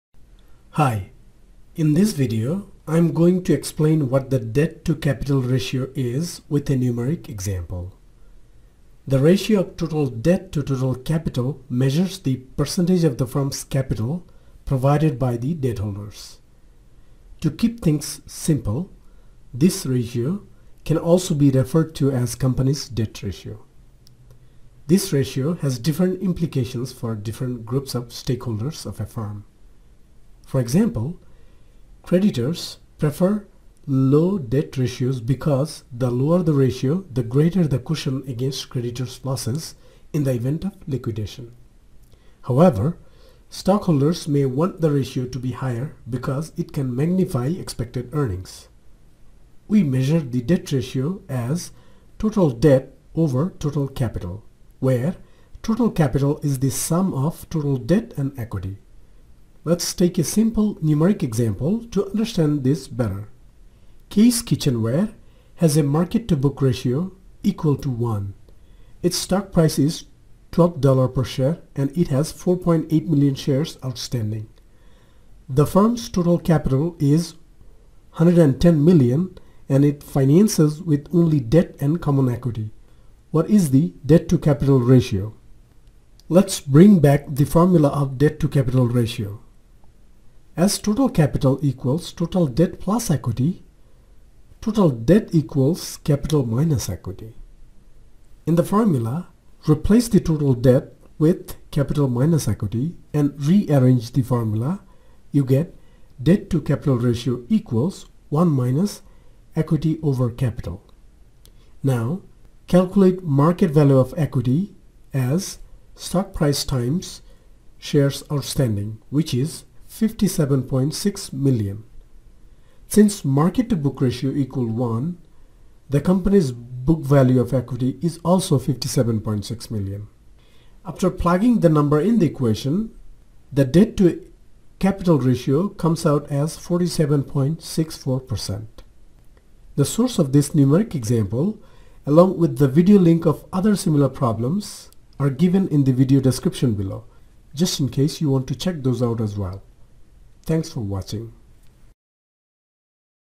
Debt to Capital Ratio | FIN-ED
fin-ed
Hi, in this video, I am going to explain what the debt-to-capital ratio is with a numeric example.

The ratio of total debt to total capital measures the percentage of the firm’s capital provided by debtholders. To keep things simple, this ratio can also be referred to as the company’s debt ratio. This ratio has different implications for different groups of stakeholders of a firm. For example, creditors prefer low debt ratios because the lower the ratio, the greater the cushion against creditors’ losses in the event of a liquidation. However, Stockholders may want the ratio to be higher because it can magnify expected earnings. We measure the debt ratio as total debt over total capital, where total capital is the sum of total debt and equity.

Kay’s Kitchenware has a market/book ratio equal to 1. Its stock price is $12 per share and it has 4.8 million shares outstanding. The firm’s total capital is $110 million and it finances with only debt and common equity. What is the debt-to-capital ratio?

Source: Fundamentals of Financial Management (Concise Edition)
Brigham and Houston
Chapter 4: Problem 4-2